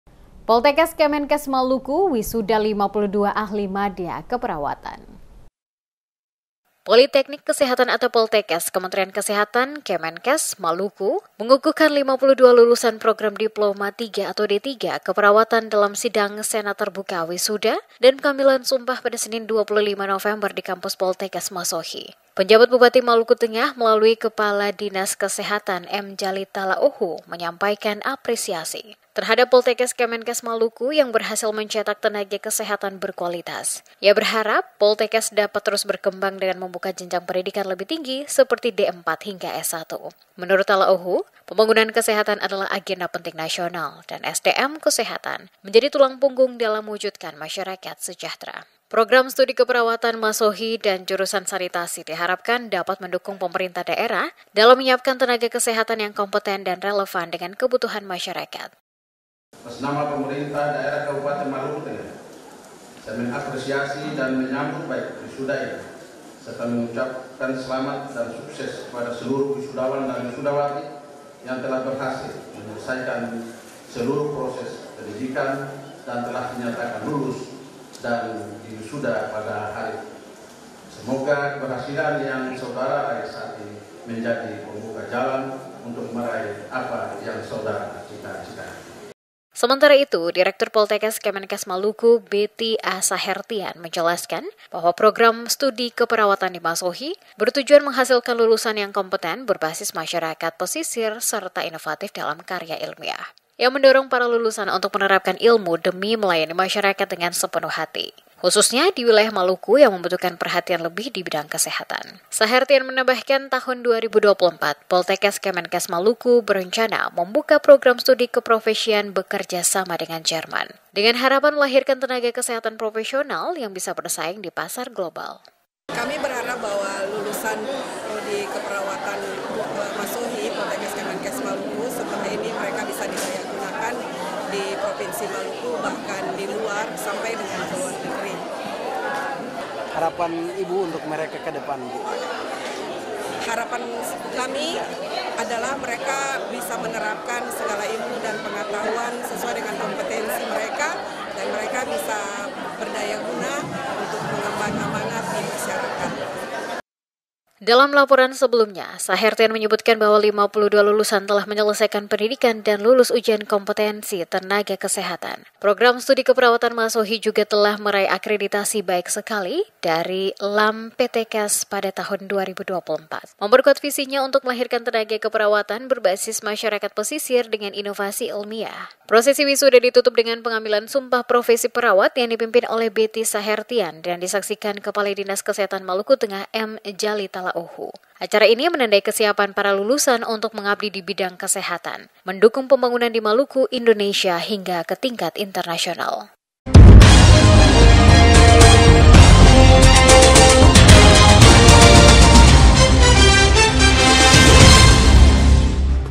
Poltekkes Kemenkes Maluku Wisuda 52 Ahli Madya Keperawatan
Politeknik Kesehatan (Poltekkes) Kementerian Kesehatan (Kemenkes) Maluku mengukuhkan 52 lulusan Program Diploma III (D-III) Keperawatan dalam Sidang Senat Terbuka Wisuda dan Pengambilan Sumpah, Senin (25/11), di Kampus Poltekkes Masohi.